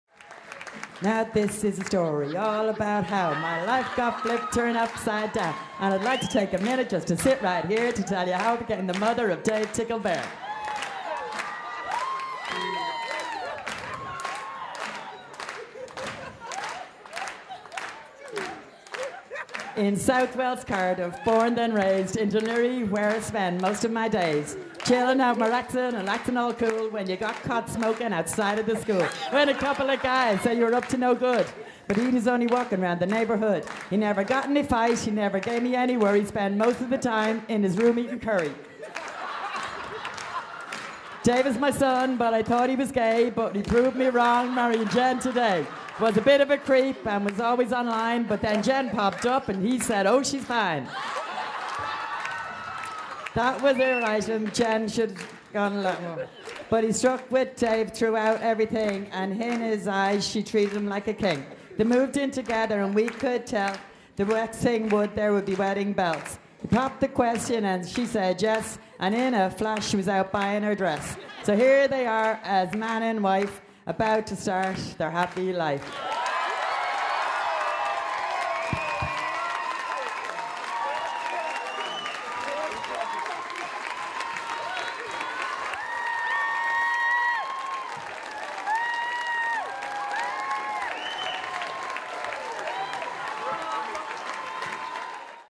Mother does a rap at son's wedding
David & Jennifers Wedding speeches Watch the look of surprise on the Grooms face as his mother break into a rap all about him in his young days.